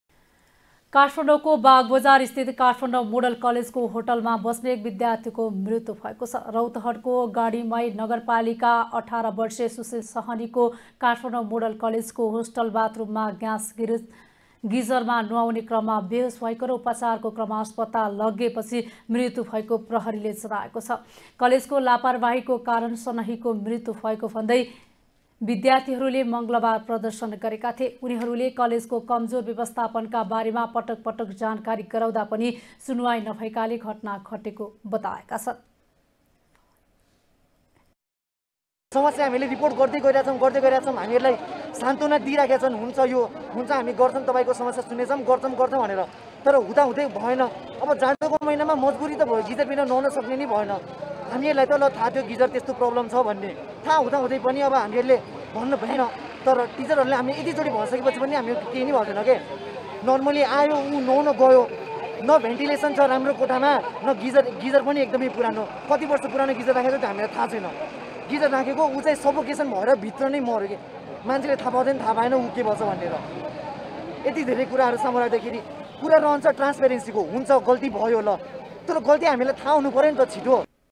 काठमाडौंको बागबजारस्थीत काठमाडौं मोडल कलेजको होस्टलमा बस्ने एक विद्यार्थीको मृत्यु भएको छ ।
© All the Audio and Video rights of this Program is owned only by Mega Television Pvt. Ltd.